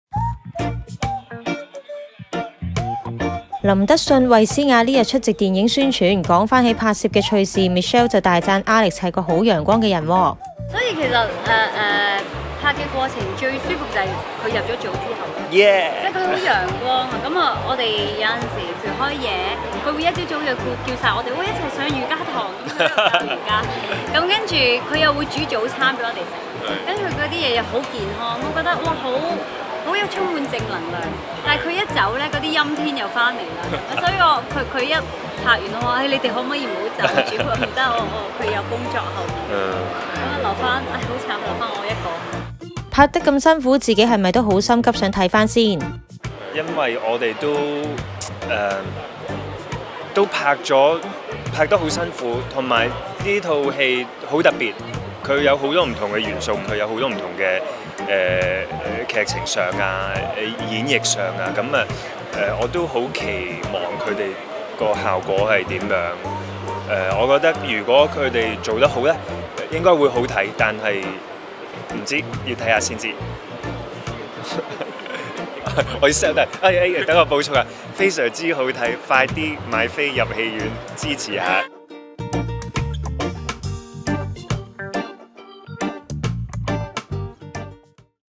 【娛樂】衛詩雅讚林德信係陽光男孩 2019-08-22
衛詩雅(Michelle)和林德信(Alex)出席電影宣傳活動，Michelle大讚他是陽光男孩。「每次開夜班，Alex一有時間就會叫大家去上瑜珈課，之後就會煮早餐俾我哋食，佢用嘅食材又健康，總之好正能量。」

bmgproductionmusic